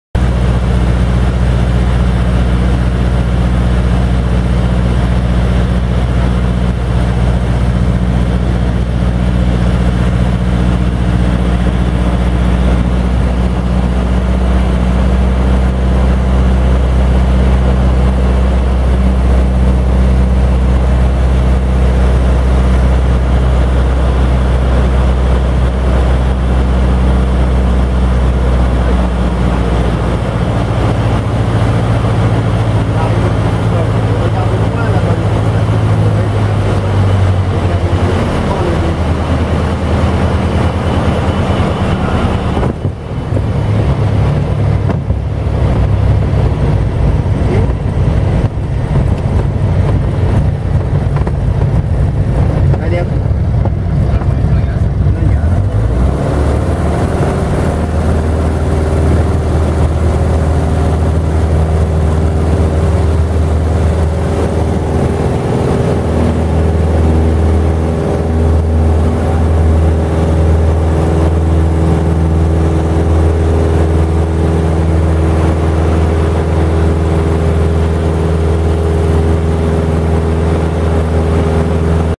Touch and Go DR 300
Toutch and GO en DR 300 (F-BSLQ) piste 09 à la Rochelle le 31/10/2015
Merci Nicolas L pour se vol ;) c'était génial ;)

Venez visiter mon site de photographie aéronautique où vous y retrouverez toutes mes vidéos ;)